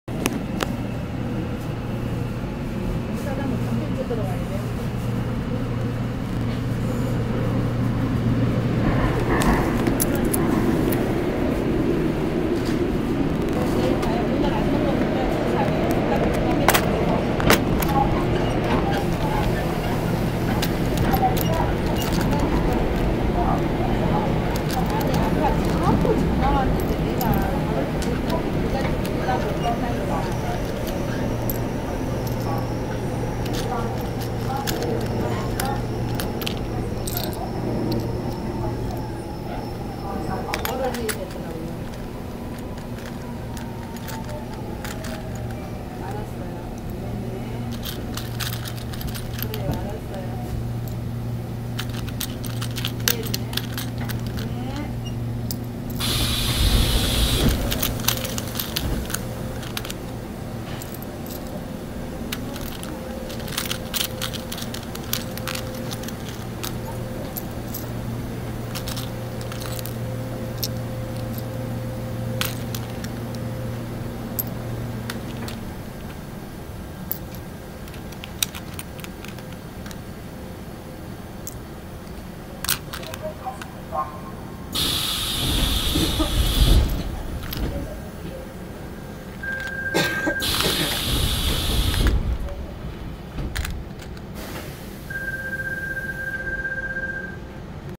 Lots of Miniature Plastic Clear Hart Cups-05
Thu 22nd Oct 2020(AH 1442 Rabi Al-Auwal).
I bought Lots of Miniature Clear Plastic Hart Cups a long time.
And this is a part of Seoul Metro Su In Bun Dang Line from Do Gok(K217) to Han Ti(K216).